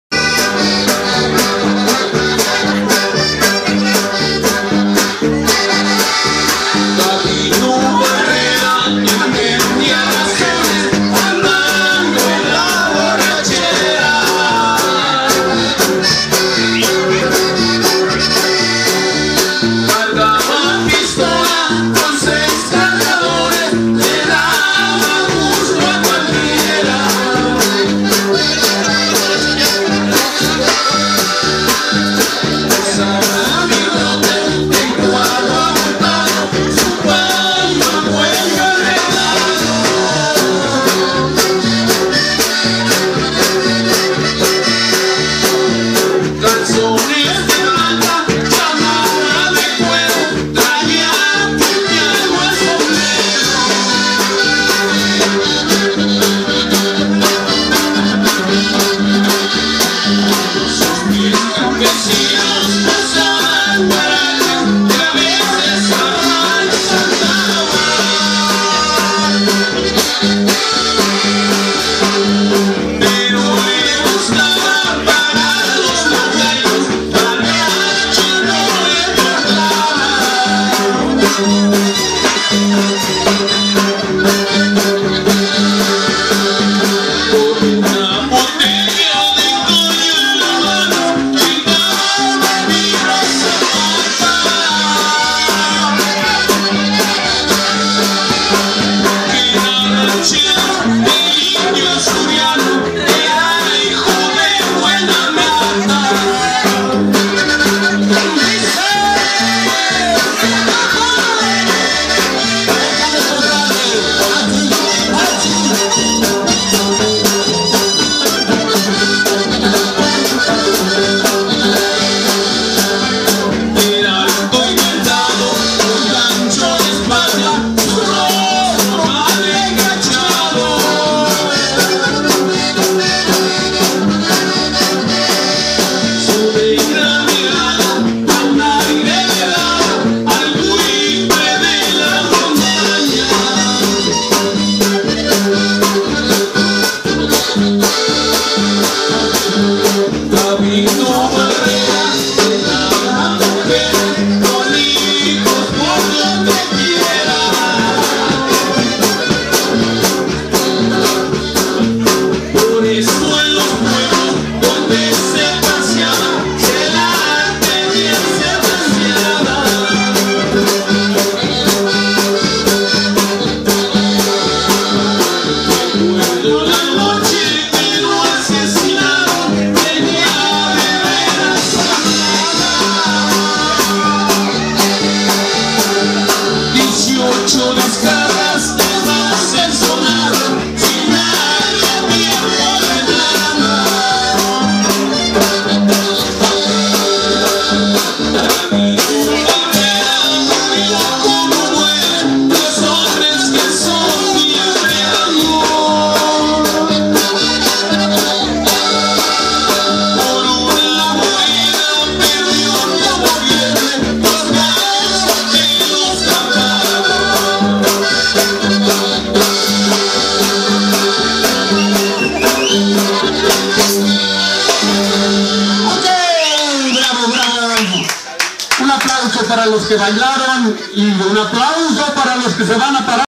Ouve toc. Gabino Ba.
Buena organización en ésta kermès, y el baile estubo en grande, eso es estar bien organizados.